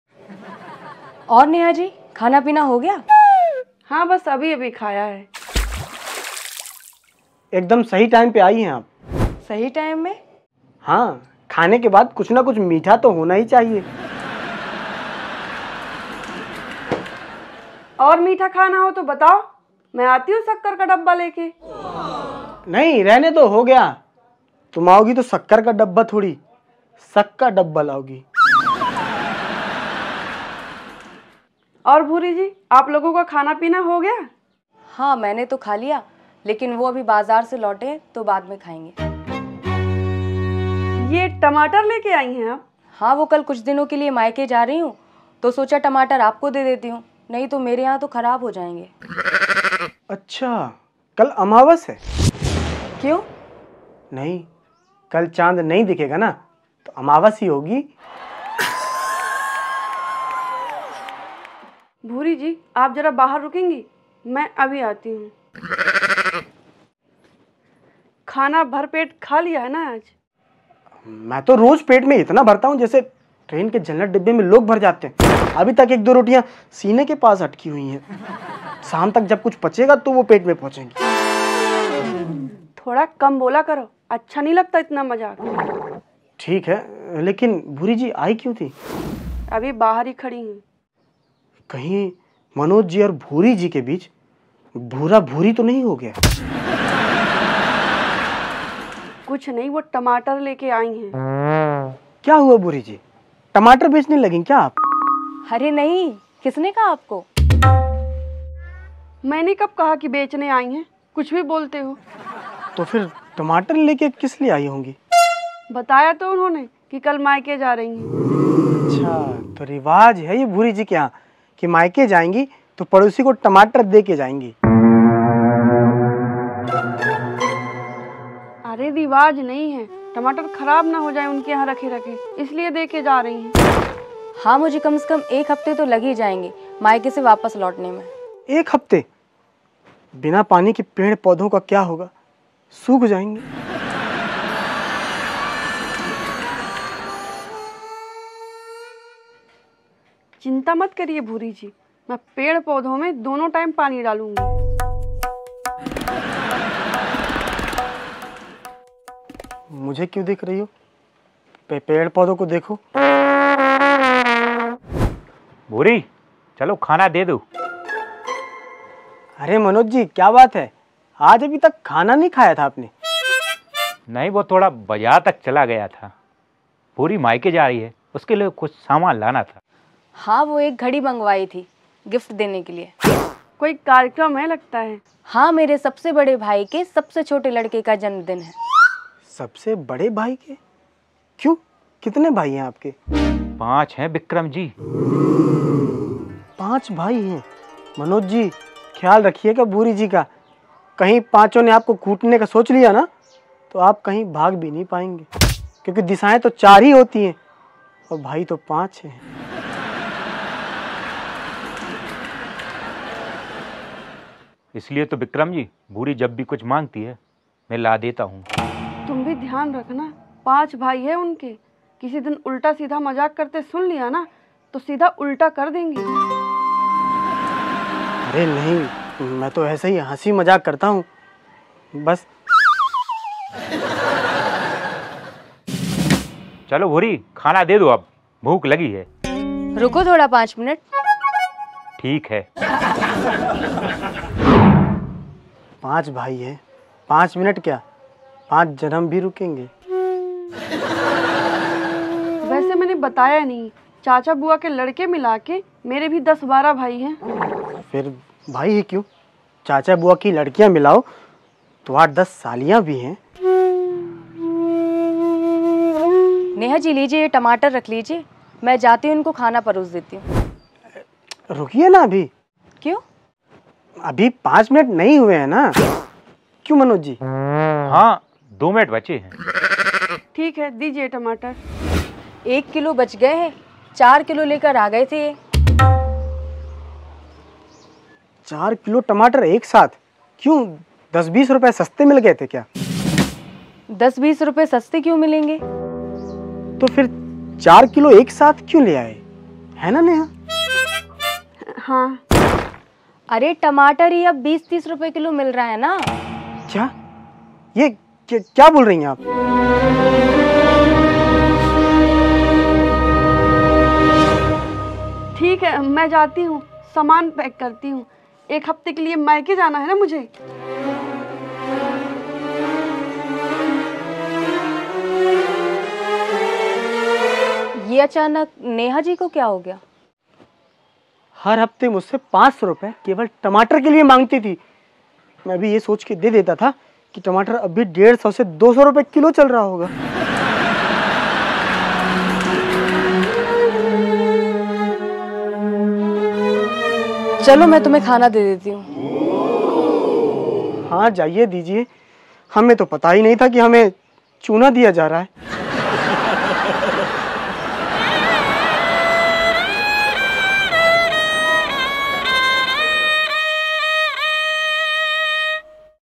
Padosan Ke Tamatar | family comedy by vikram bagri | new comedy video 2023
hello friends

ye raha naya comedy video vikram neha ki family ka

vikram ki padosan biwi aur tamatar ke beech kya hai kahani

aaiye dekhte hain

ab is video me aapko maja to aayega kyunki hai ye pure family comedy

to ise dekhiye aur bataiye ki kaisa laga

family comedy video